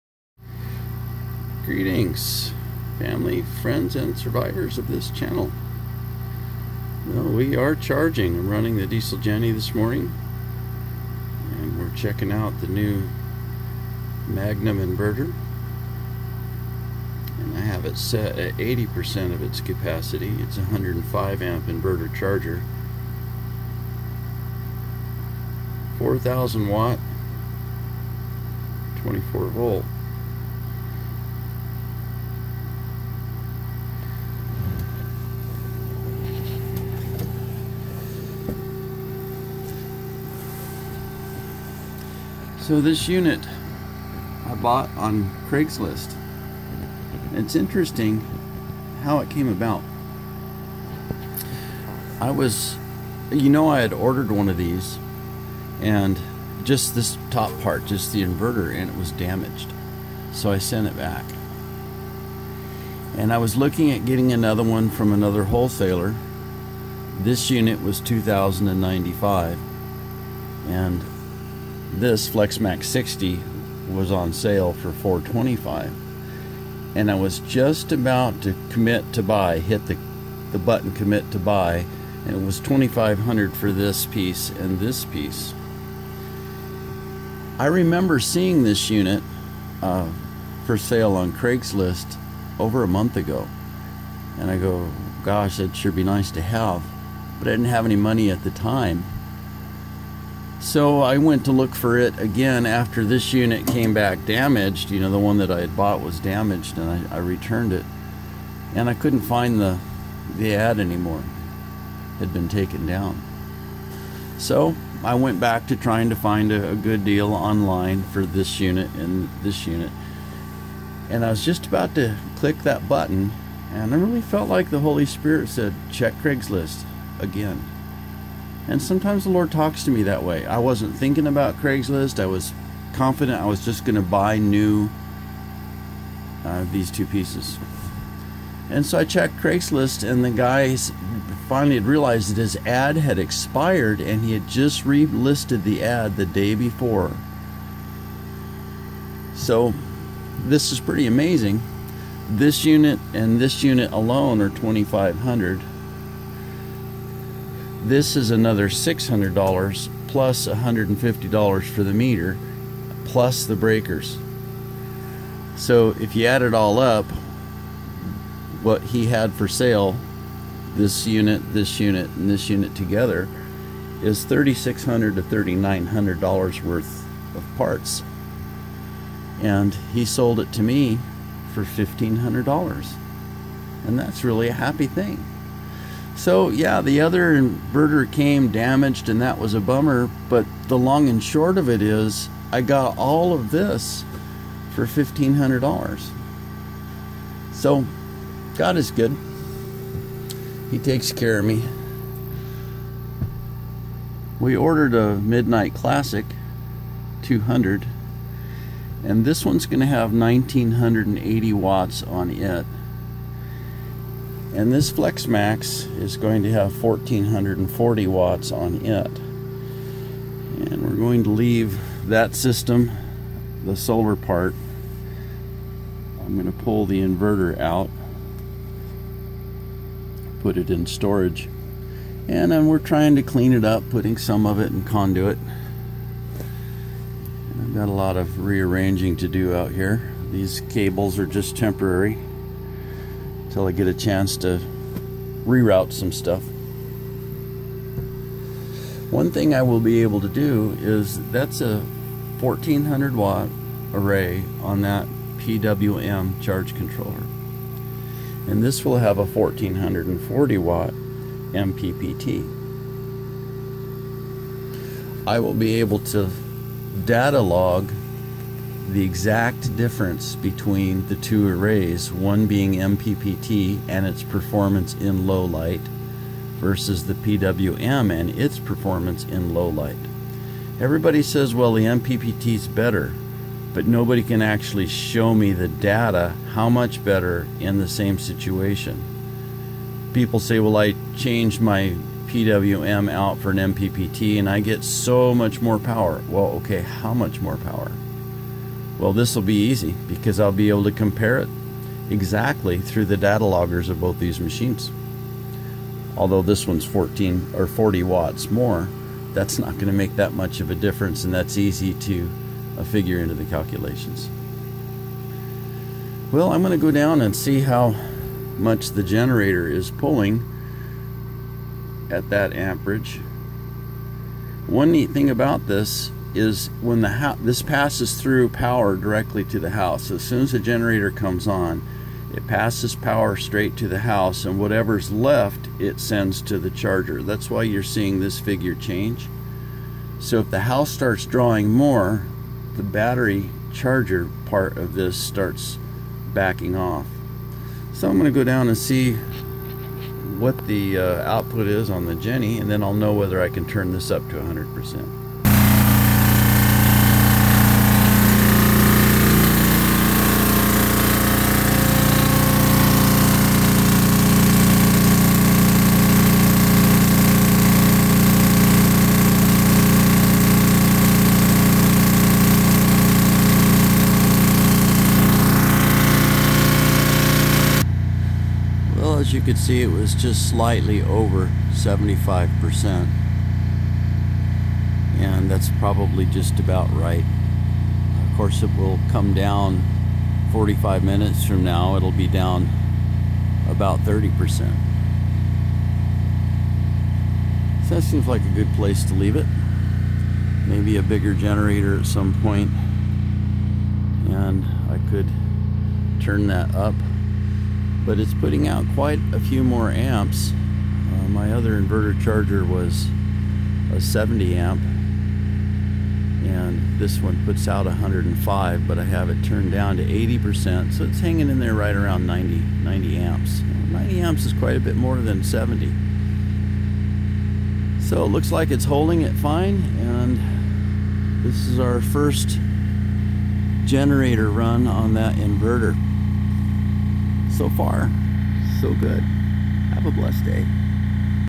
The off grid solar homestead gets a new inverter
It is going to take a few months to get the 18 new solar panels up and running. This inverter has a 105 amp PFC  battery charger in it so the generator time is much shorter. The pure sinewave is very nice, the celling fans are whisper quiet now.